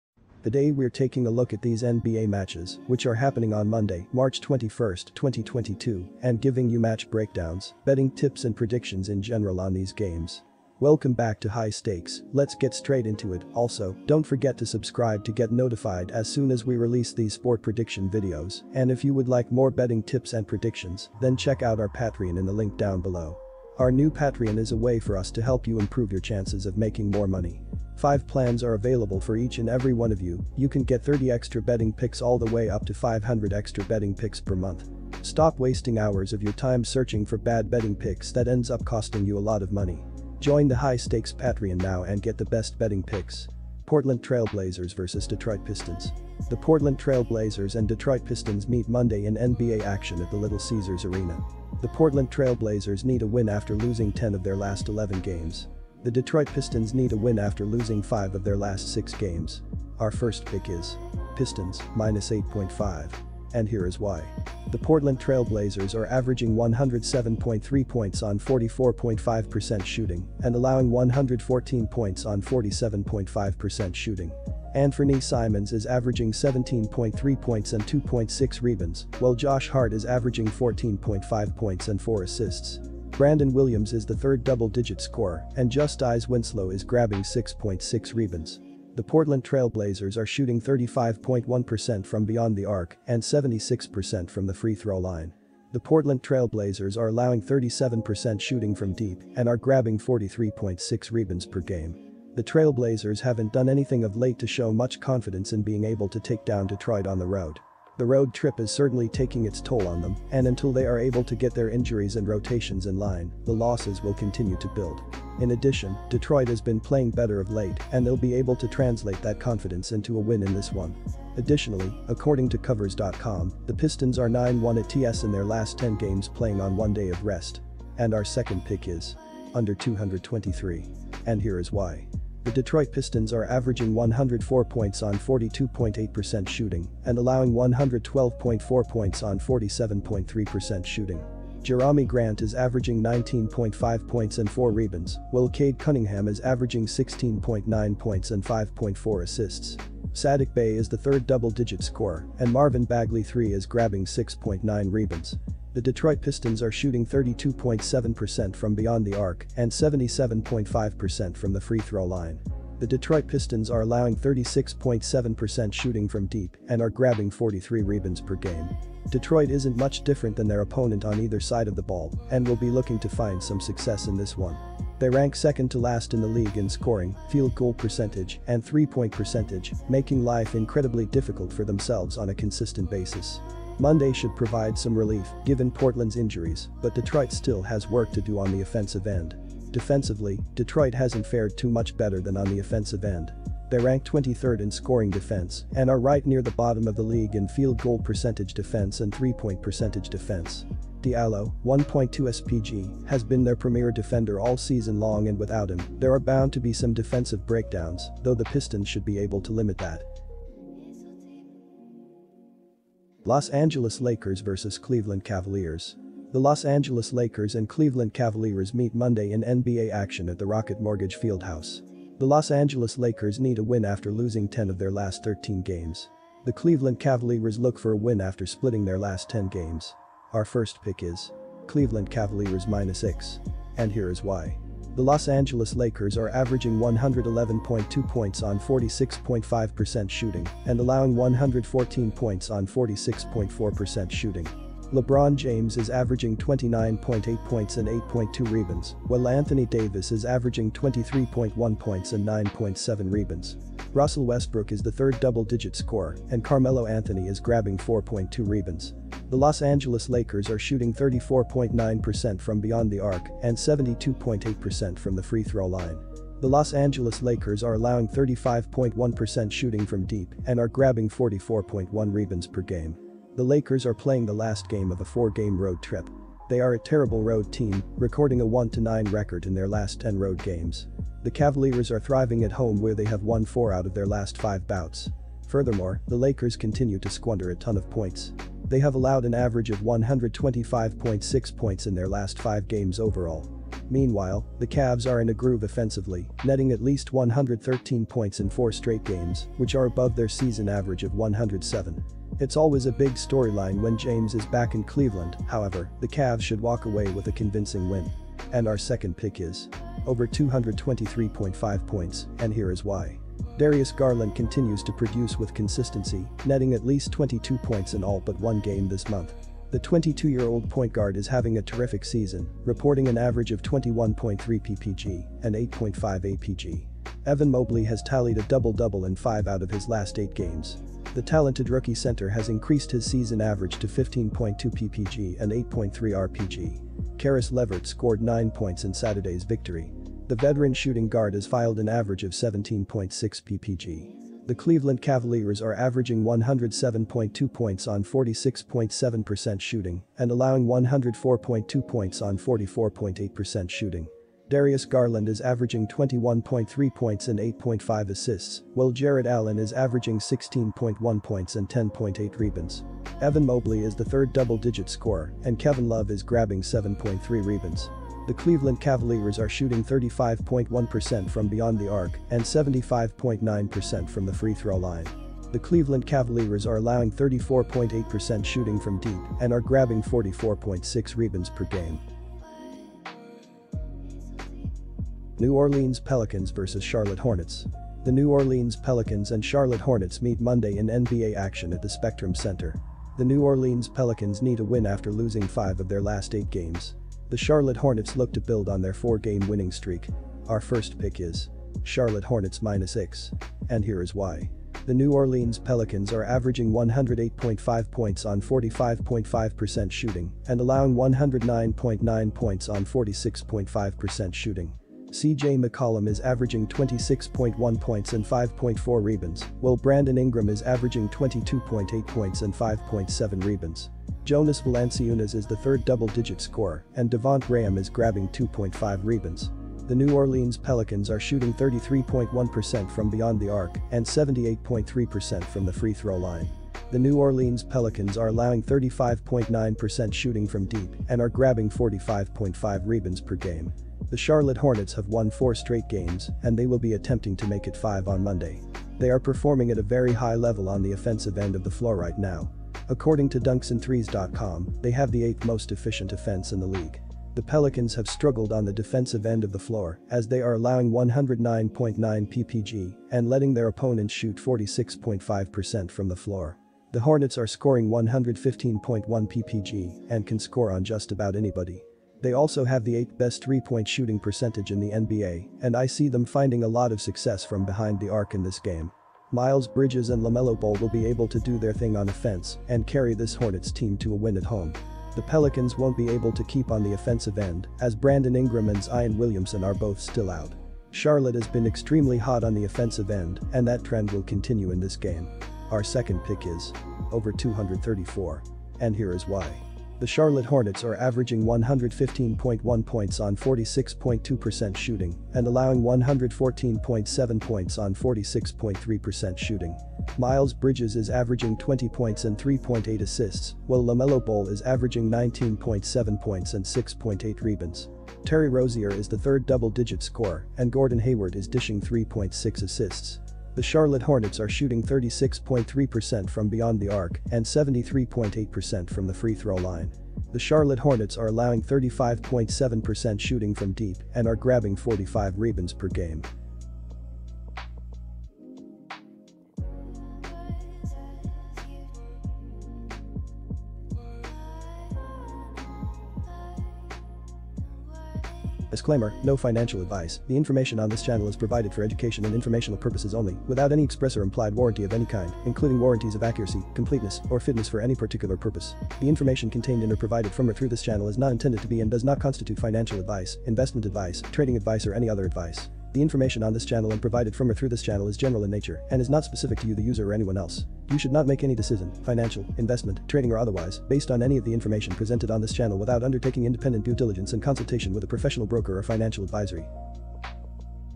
Free NBA Picks and Predictions for Today 3/21/22 Best NBA Betting Tips
Portland Trail Blazers vs Detroit Pistons
Los Angeles Lakers vs Cleveland Cavaliers
New Orleans Pelicans vs Charlotte Hornets